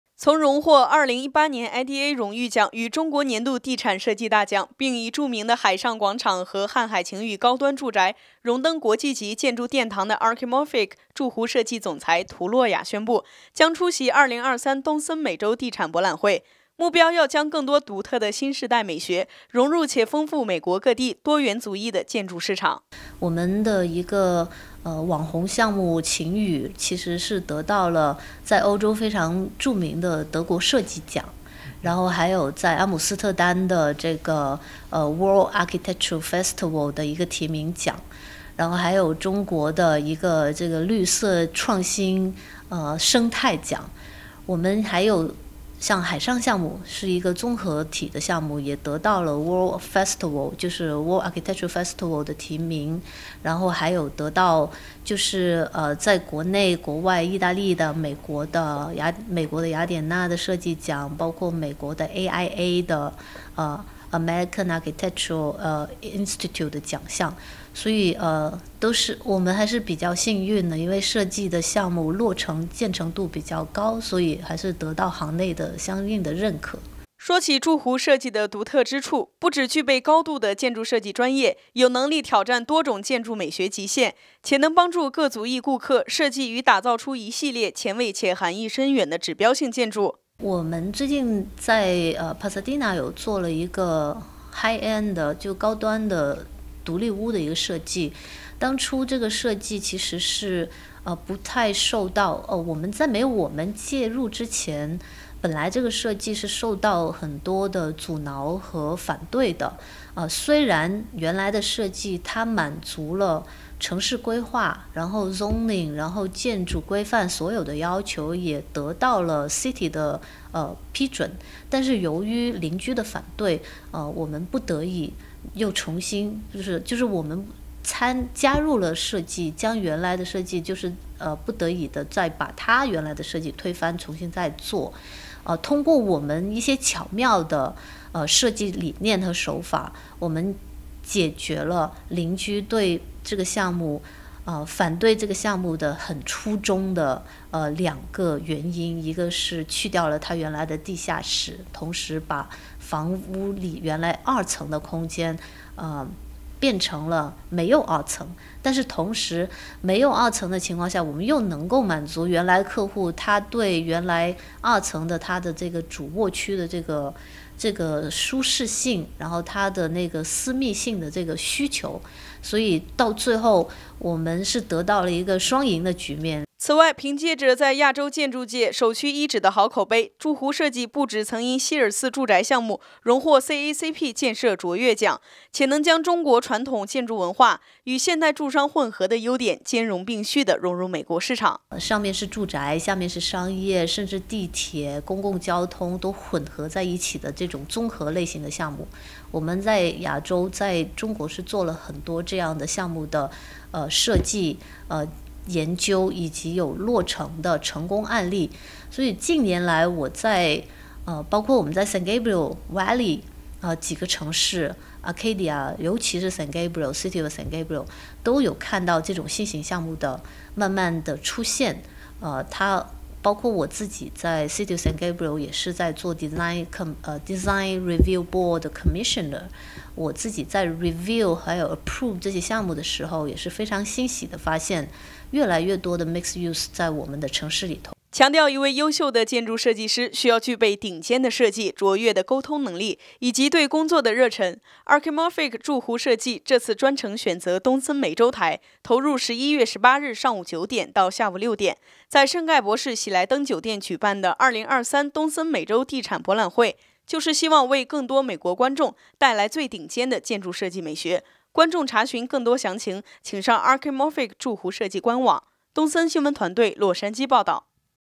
Archimorphic築弧設計獲獎無數 出席東森美洲地產博覽會
一向以專業且獨特的建築設計風格，分別在全球創下好口碑的「築弧設計」，最近特別宣布，正式加入"2023東森美洲地產博覽會"，要幫助更多來賓，建造出獨一無二的傳奇建築。

曾榮獲"2018年IDA榮譽獎"，與"中國年度地產設計大獎"，並以著名的"海尚廣場"和"翰海晴宇高端住宅"，榮登國際級建築殿堂的"Archimorphic、築弧設計"總裁，涂洛雅宣布，將出席"2023東森美洲地產博覽會"，目標要將更多獨特的新世代美學，溶入且豐富美國各地、多元族裔的建築市場。

說起"築弧設計"的獨特之處，不只具備高度的建築設計專業，有能力挑戰多種建築美學極限，且能幫助各族裔顧客，設計與打造出一系列、前衛且含意深遠的指標性建築 。

此外，憑藉著在亞洲建築界，首屈一指的好口碑，「築弧設計」不只曾因"希爾茨住宅項目，榮獲"CACP建設卓越獎"，且能將傳統中國建築文化，與現代住商混和的優點，兼容並蓄的融入美國市場。

強調一位優秀的建築設計師，需要具備頂尖的設計、卓越的溝通能力，以及對工作的熱忱。"Archimorphic、築弧設計"，這次專程選擇東森美洲台，投入11月18日上午九點到下午六點，在"聖蓋博市"喜來登酒店舉辦的"2023東森美洲地產博覽會"，就是希望為更多美國觀眾，帶來最頂尖的建築設計美學。觀眾查詢更多詳情，請上"Archimorphic、築弧設計"官網。